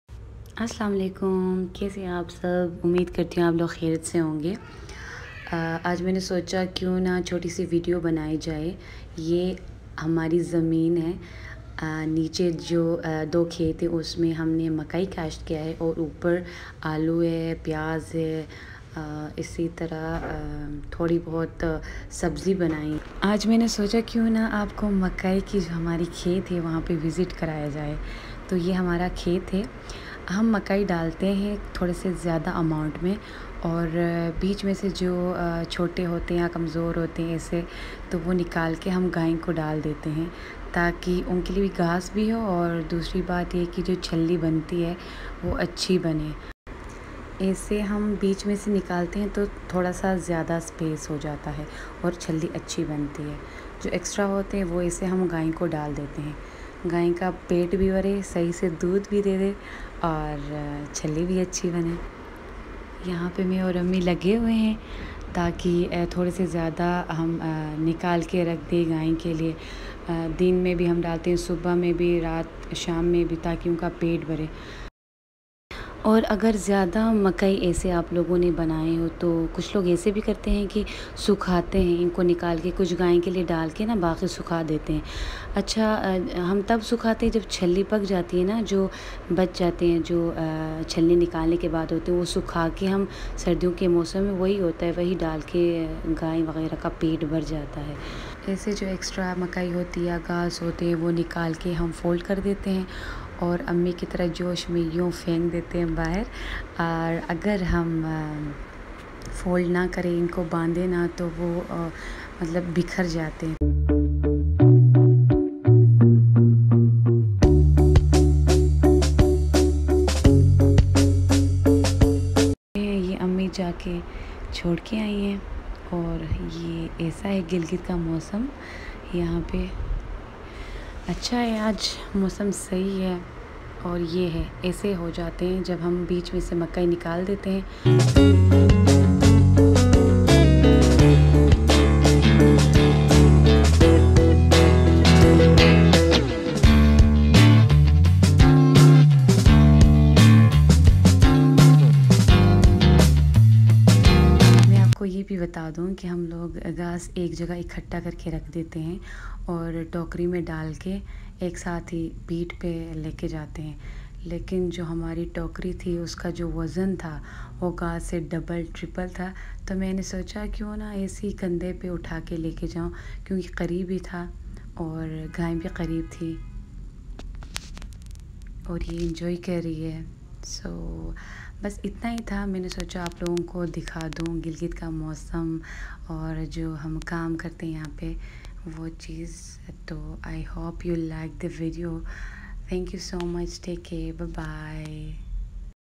Life with Nature | Collecting corn leaves | Village life
We collect corn leaves for cow and goats, these all are the real activities of the people from gilgit.